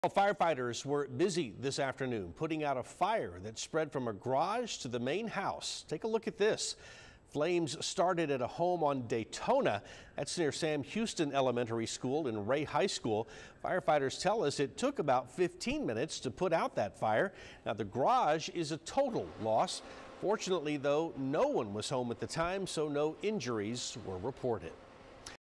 Fire crews put out garage fire on Daytona Drive
The garage was deemed a total loss. Fortunately, no one was home at the time, so no injuries were reported.